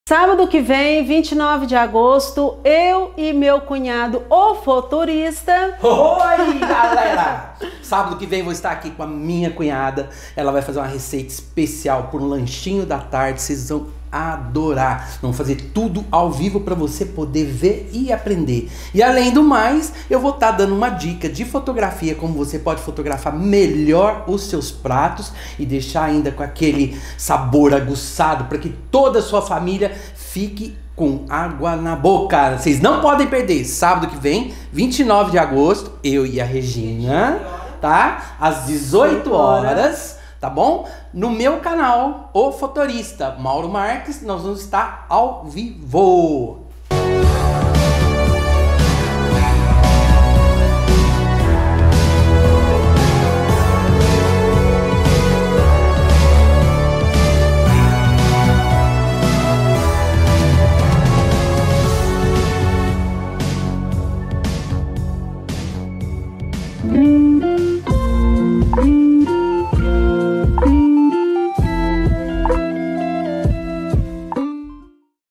Live Sábado 29/08 - 18h
Farei a live que prometi, junto com minha cunhada mostrando a montagem de um prato e a finalização com dicas de fotografias gastronômicas. não percam.^